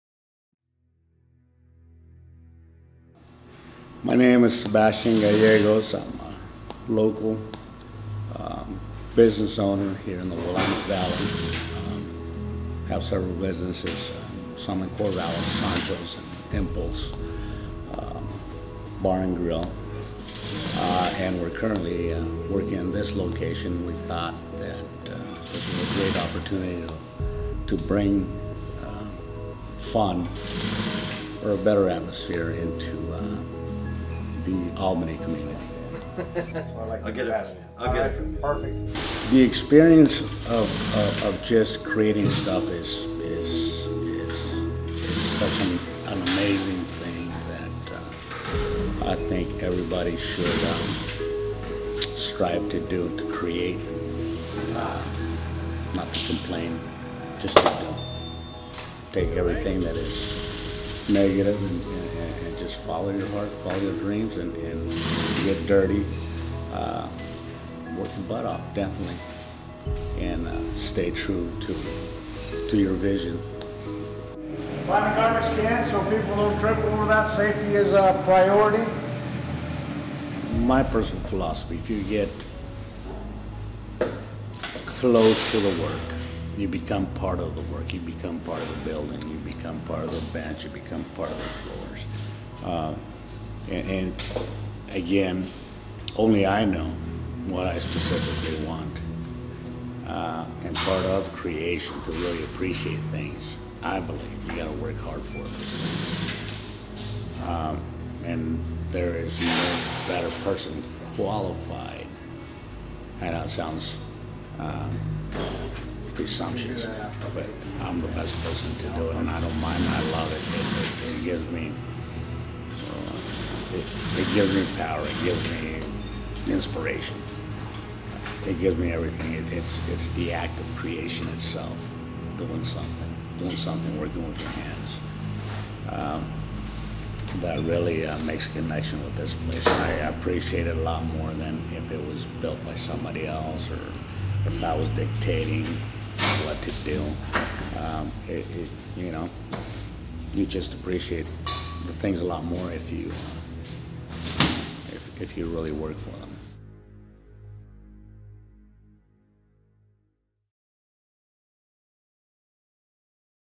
Entrepreneur Spotlight //Sebastian Gallegos
Here's a quick  interview we shot with Corvallis entrepreneur Sebastian Gallegos on his upcoming nightclub located in Albany, Ore.